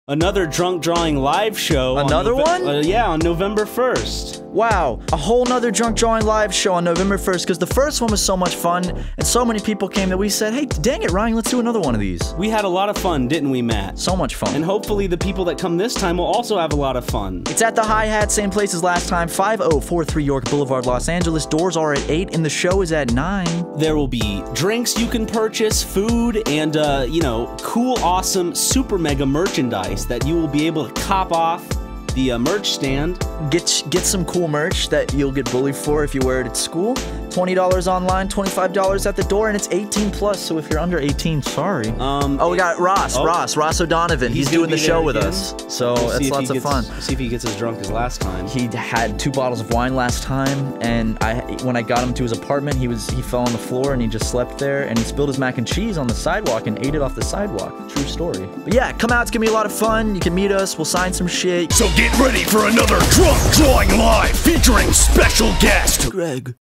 Drunk Drawing LIVE - NOVEMBER SHOW
We're doing a 2nd Drunk Drawing Live show November 1st!

Come drink with us, watch us get blacked out and draw things you suggest, we'll even pull some of you up to do portraits. Cool merch and food! See you there!

18+
WHEN: November 1st, doors at 8:00 PM, show at 9:00 PM
WHERE: The Hi Hat - 5043 York Blvd, Los Angeles
WHY: ?